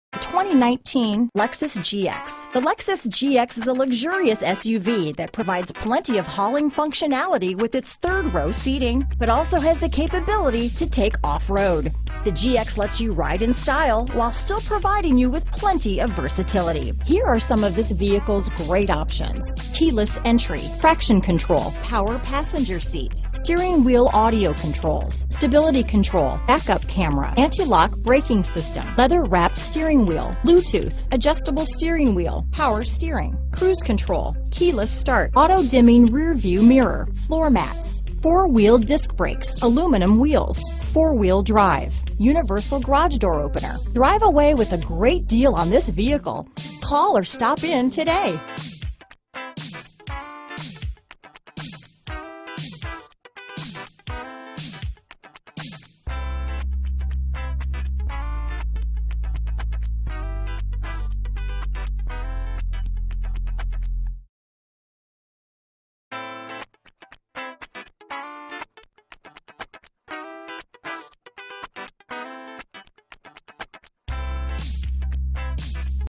2019 Lexus GX Duluth, Johns Creek, Buford, Suwannee, Lawrenceville, GA G190227
Black Onyx New 2019 Lexus GX available in Duluth, Georgia at Hennessy Lexus of Gwinnett. Servicing the Johns Creek, Buford, Suwannee, and Lawrenceville, GA area.
2019 Lexus GX 460 Premium - Stock#: G190227 - VIN#: JTJBM7FX1K5214069